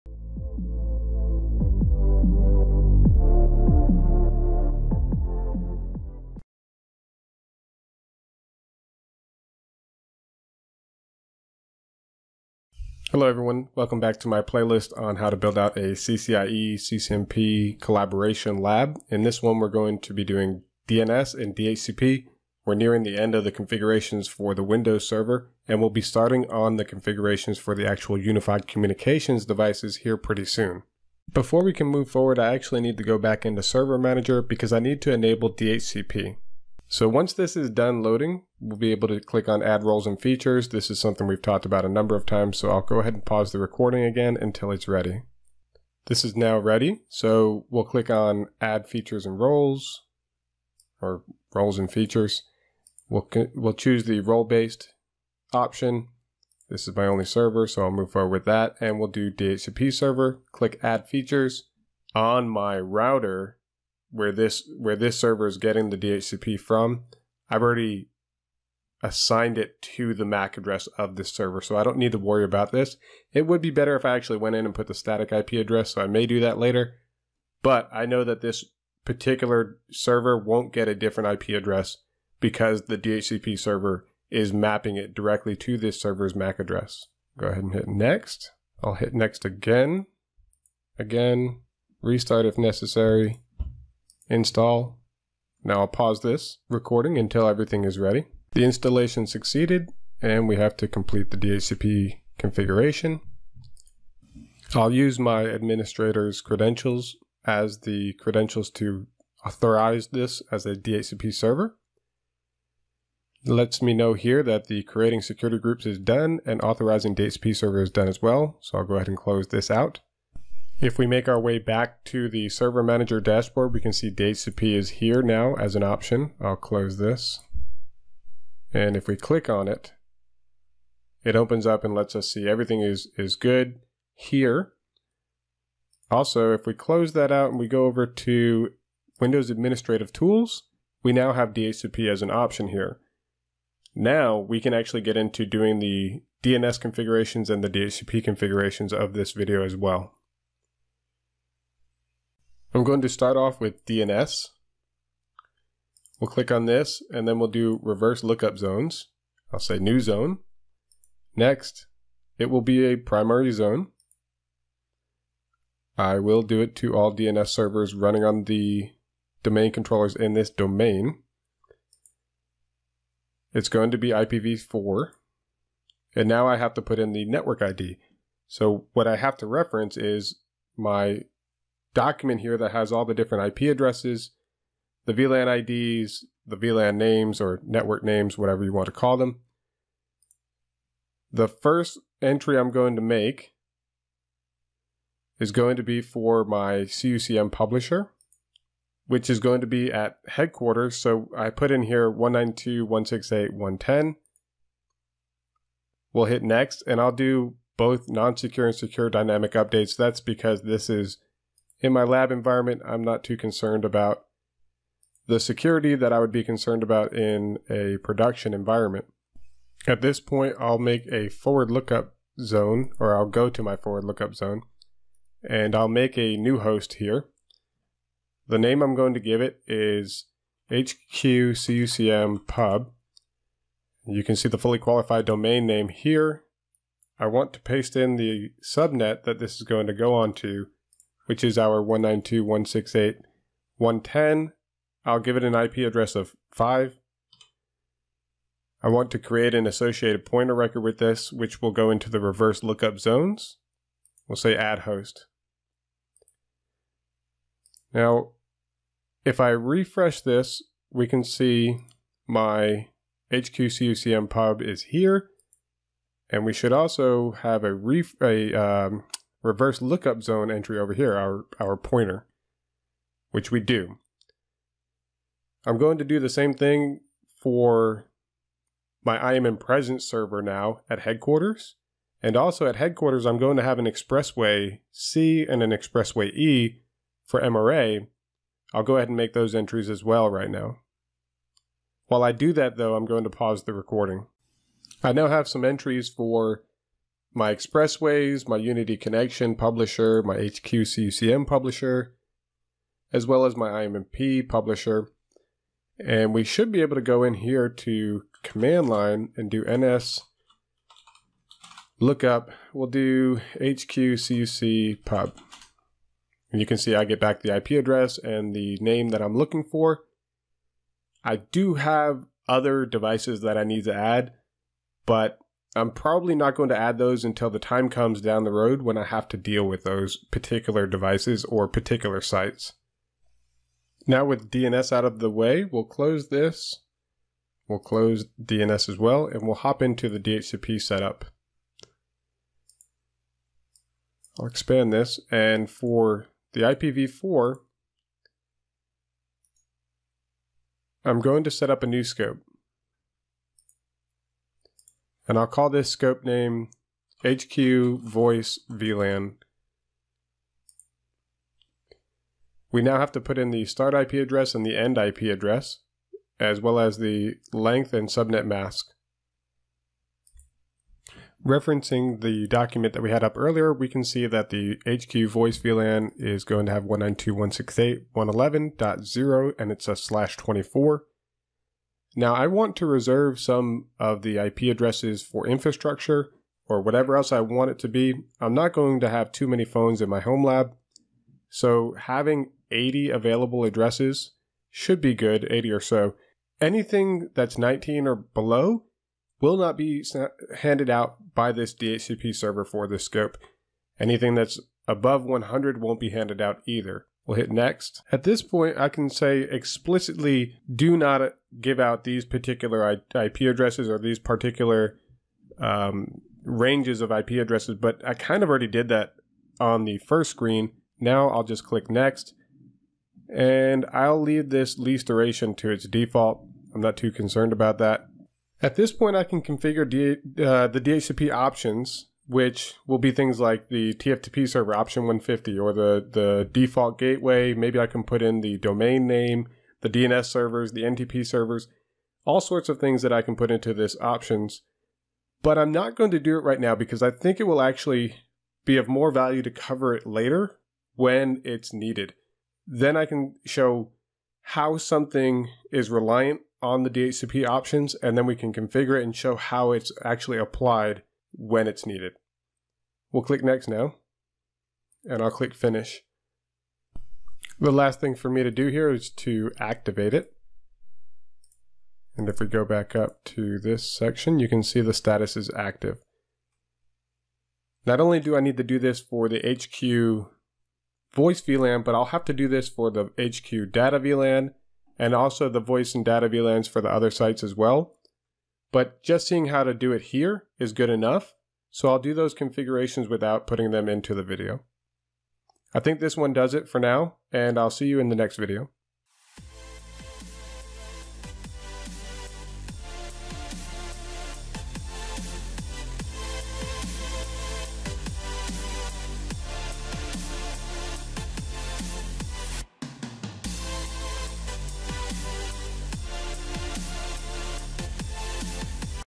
BUILD A LAB - CCNP/CCIE COLLAB - Config Windows Server Part 4
How to enable Certificate Services, Lightweight Directory Services, and configure the NTP client on Windows Server 2019